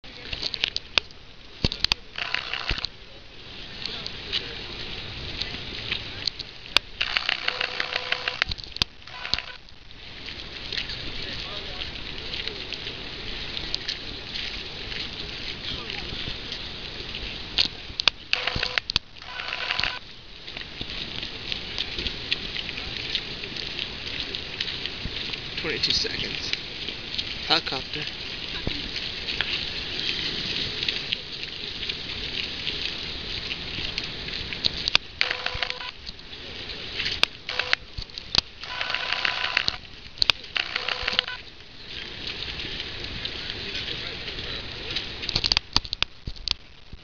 2012 Edmonton Canada Day Fireworks - Video 1
Mine and Michelle's first fireworks in Edmonton, with our friends Elicia and Johnathan. Walking back to the bus on our way back home.